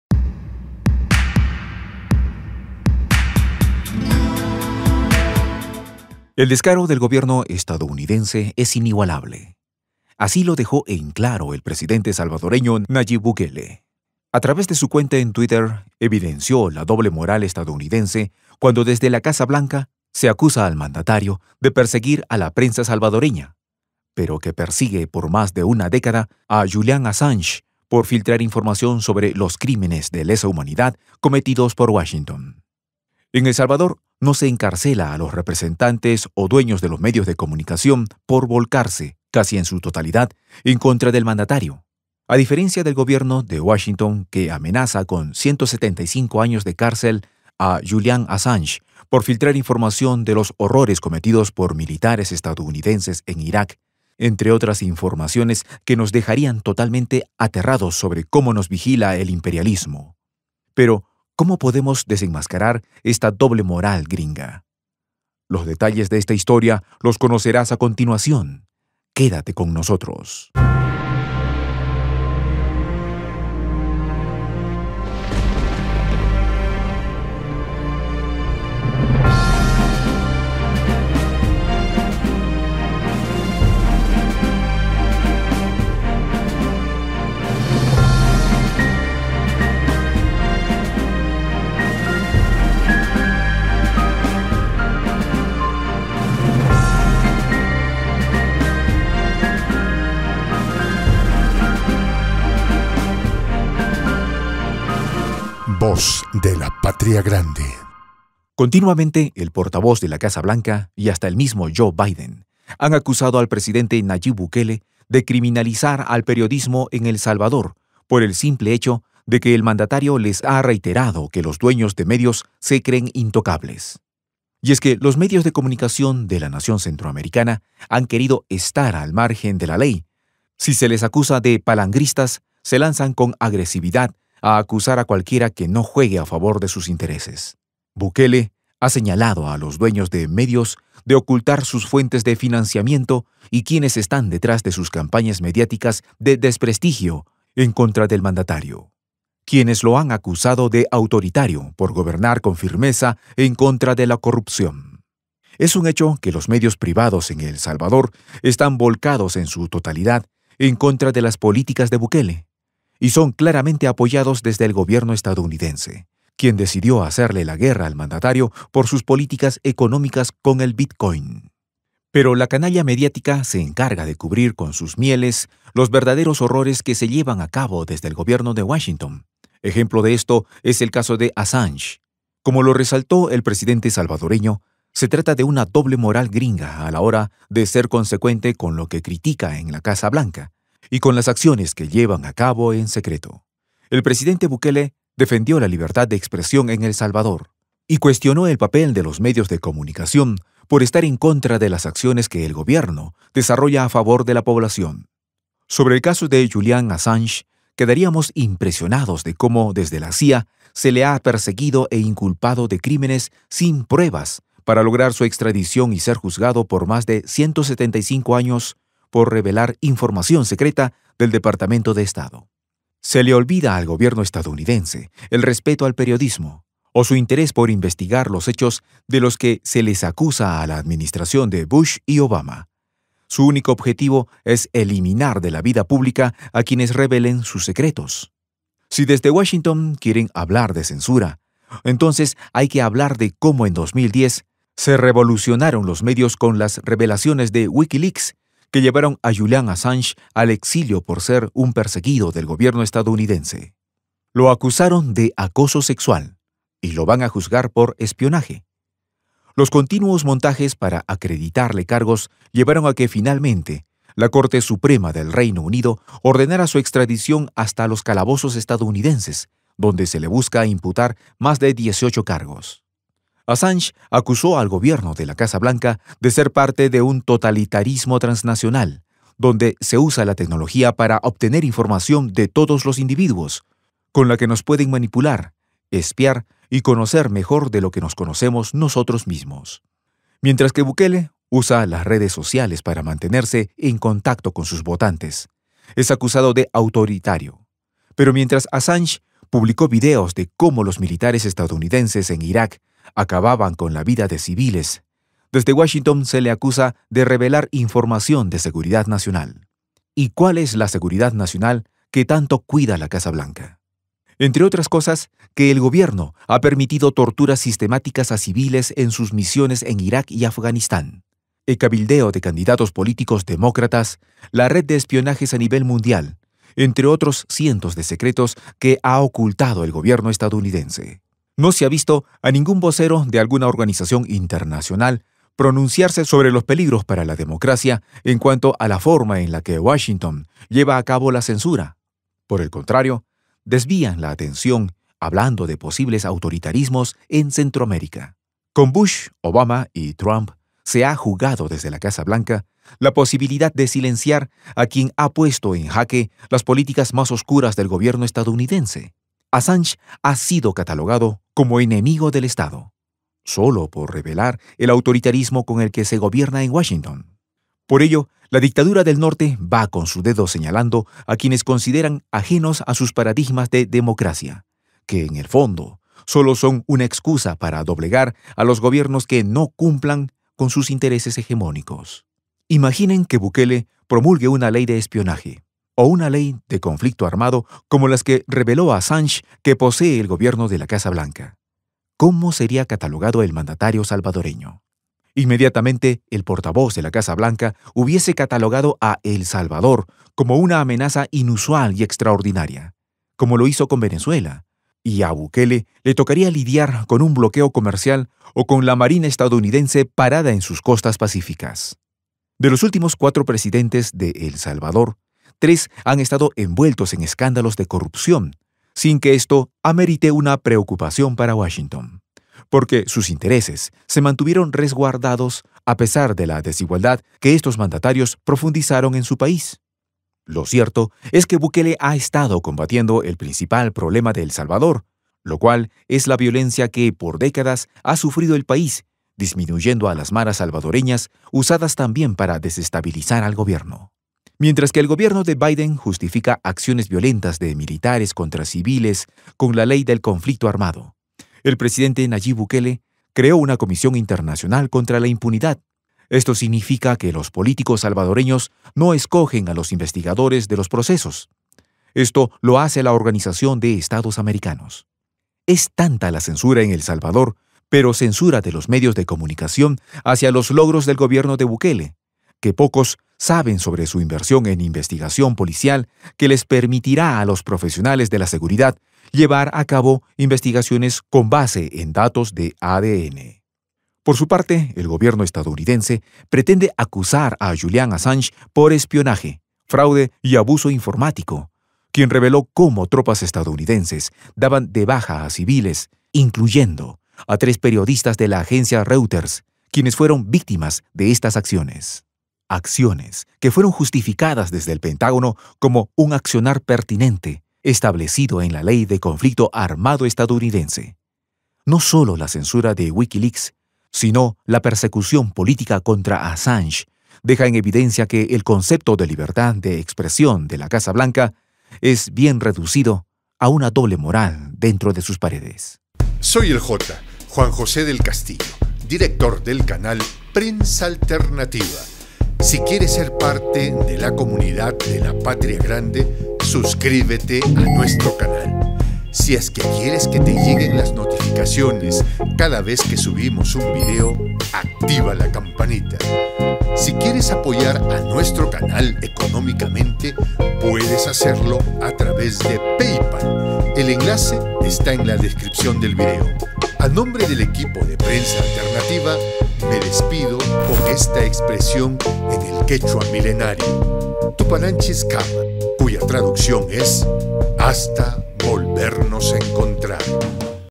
Extradición de Assange deja al descubierto doble moral gringa!
El presidente de El Salvador Nayib Bukele, aseguró que existe una doble moral estadounidense sobre el trato que se le da a la prensa. La extradición de Julian Assange es la muestra de la censura que existe desde la Casa Blanca.

Jefe de Prensa: Enzo Anchante